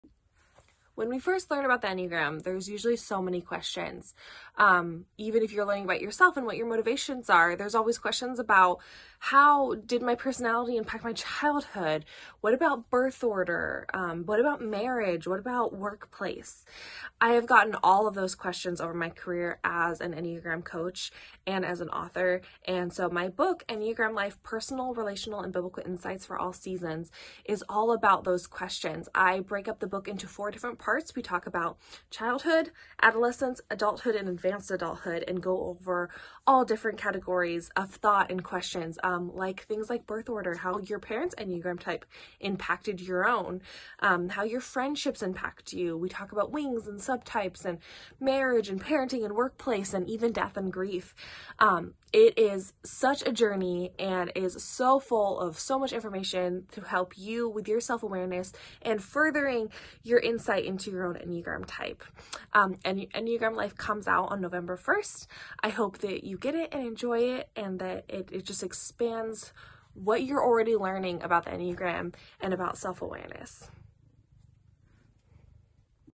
Enneagram Life
Author Elisabeth Bennett introduces her new book, Enneagram Life.